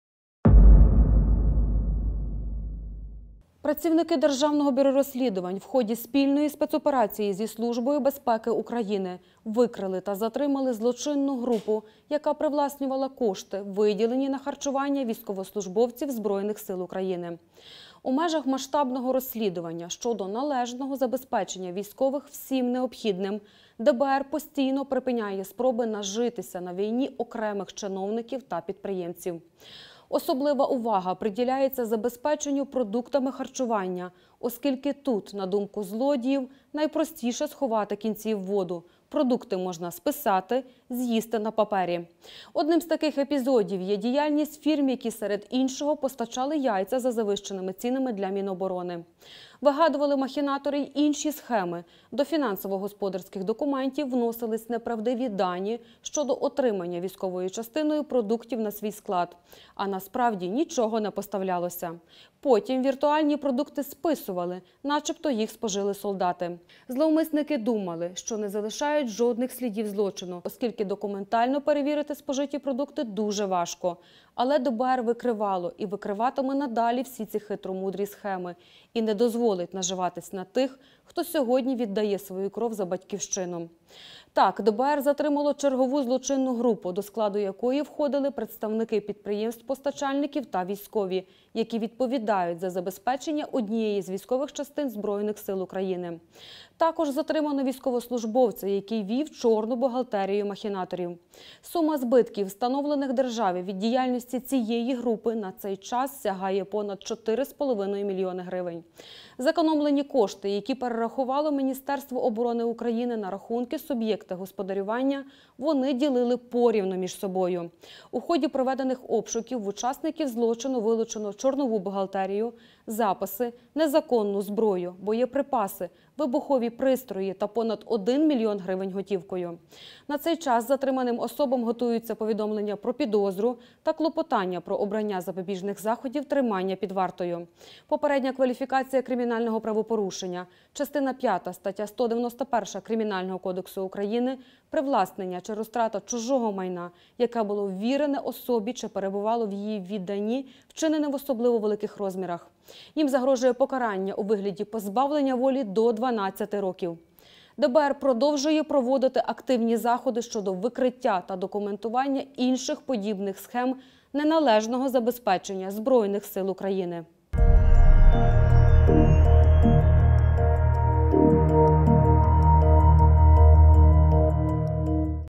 Вони життя за вас, а ви їм ЯЙЦЯ ПО 17 ГРН? ДБР РОЗКРИЛО СХЕМУ злодіїв, що наживались НА ЗСУ
Працівники Державного бюро розслідувань у ході спільної спецоперації зі Службою безпеки України викрили та затримали злочинну групу, яка привласнювала кошти, виділені на харчування військовослужбовців Збройних Сил України.

На цей час затриманим особам готуються повідомлення про підозру та клопотання про обрання запобіжних заходів у вигляді тримання під вартою.